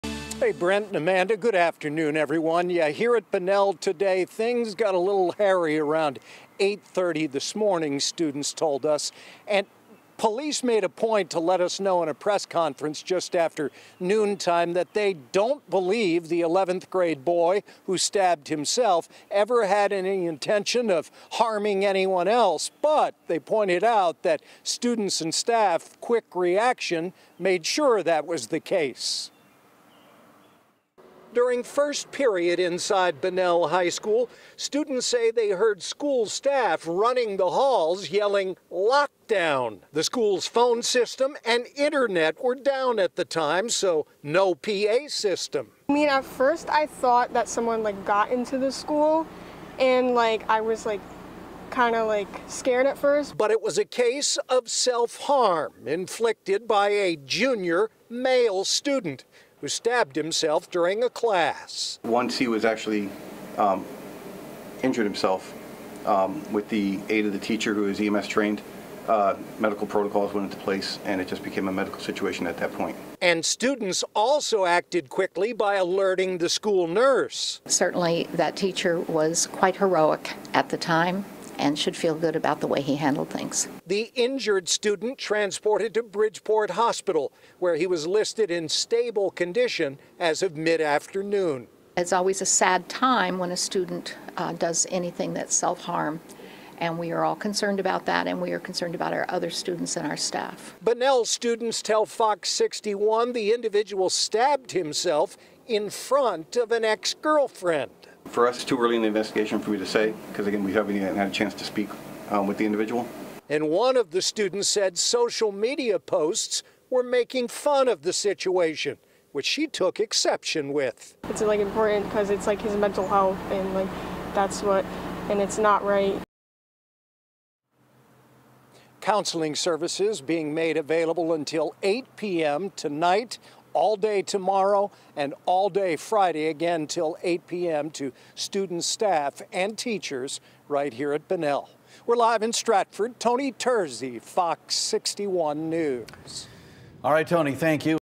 Student at Bunnell High stabs himself
School went into lockdown